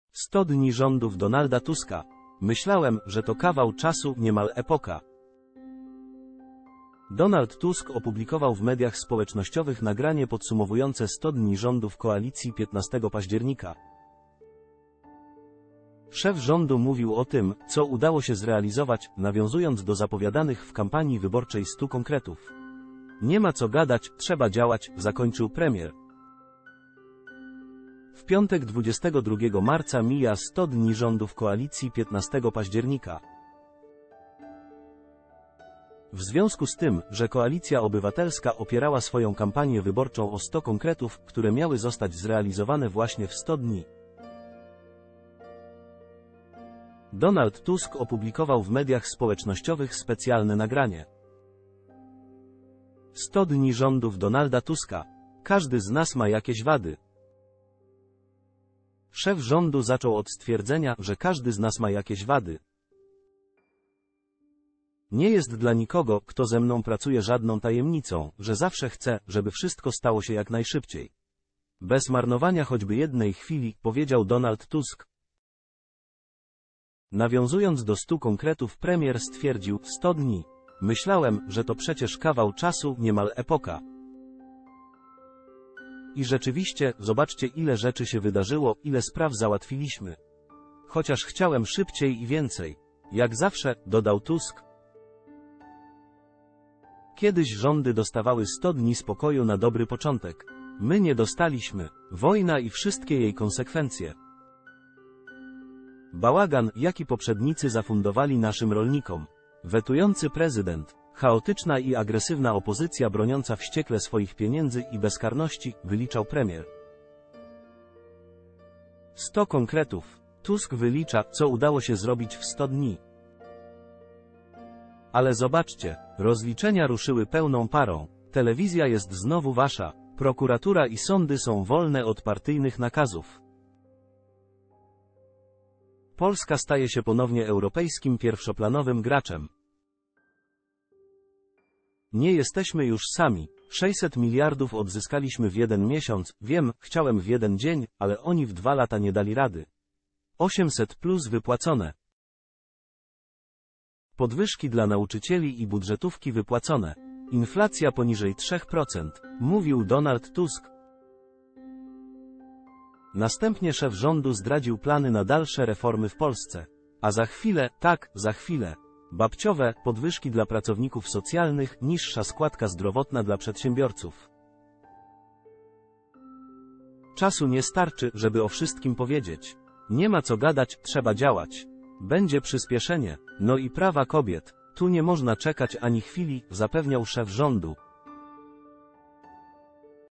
100 dni rządów Donalda Tuska ?''Myślałem, że to kawał czasu, niemal epoka''
100 dni rządów Donalda Tuska ? ''Myślałem, że to kawał czasu, niemal epoka''